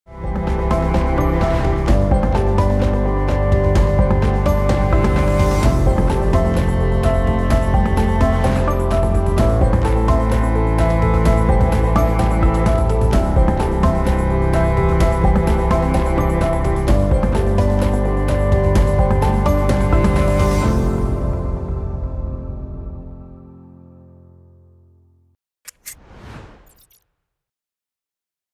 Siha Residence | President C - Villa Trailer
Pleasure to introduce President C - Villas, a new luxurious living residence developed by SIHA Property CO.,LTD. Built-In a long Hun Sen Blvd (60M Road) Heart of Phnom Penh City.

Siha Residence is Private yet convenient, facilitated, with a house like-resort pampered with luxurious modern tropical design.

We are excited to offer video production services for online and digital platforms - no matter the project size or type.
📞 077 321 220 | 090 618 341